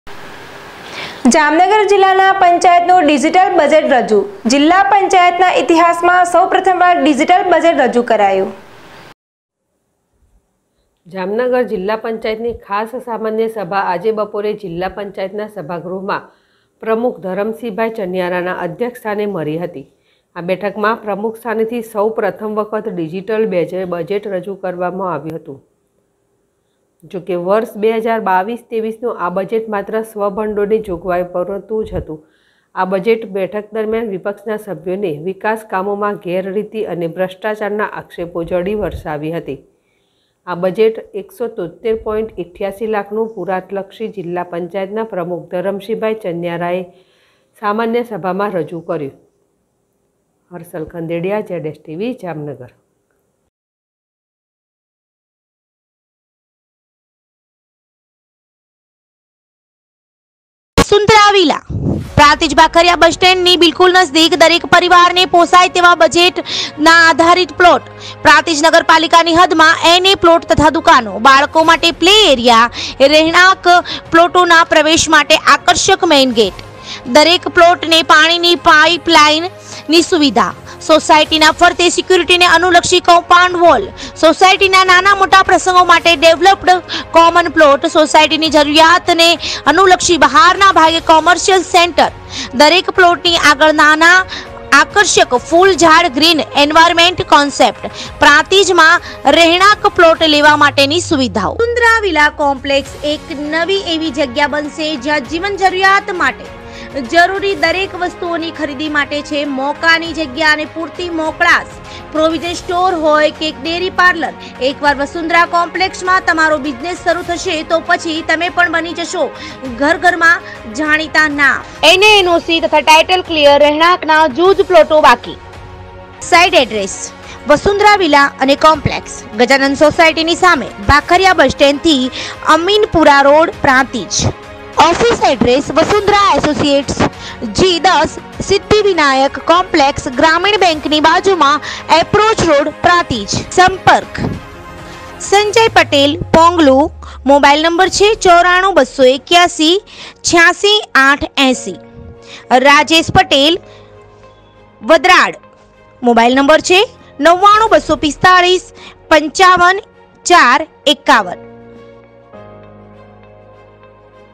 જામનગર જિલ્લા પંચાયતનું ડિજિટલ બજેટ રજૂ. જિલ્લા પંચાયતના ઇતિહાસમાં સૌપ્રથમવાર ડિજીટલ બજેટ રજૂ કરાયું
જામનગર જિલ્લા પંચાયતનું ડિજિટલ બજેટ રજૂ. જિલ્લા પંચાયતના ઇતિહાસમાં સૌપ્રથમવાર ડિજીટલ બજેટ રજૂ કરાયું//ZSTVNEWS//